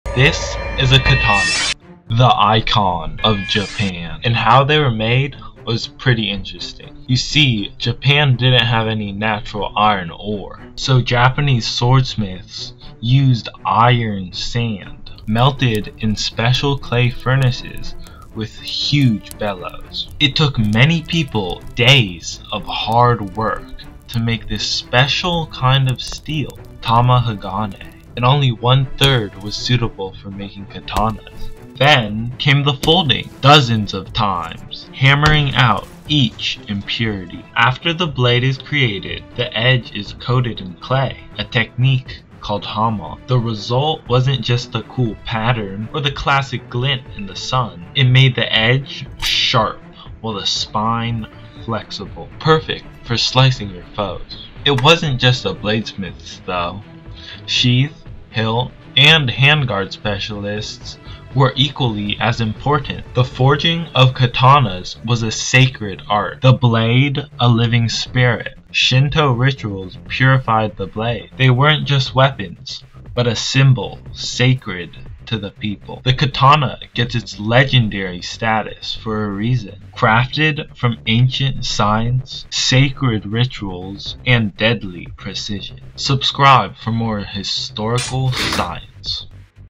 The Science Behind Japan’s LEGENDARY Katana
Discover the fascinating process behind the forging of Japan’s legendary katana — from melting iron sand in Tatara furnaces to folding Tamahagane steel and the sacred Shinto rituals that turned these blades into symbols of honor. ⚔️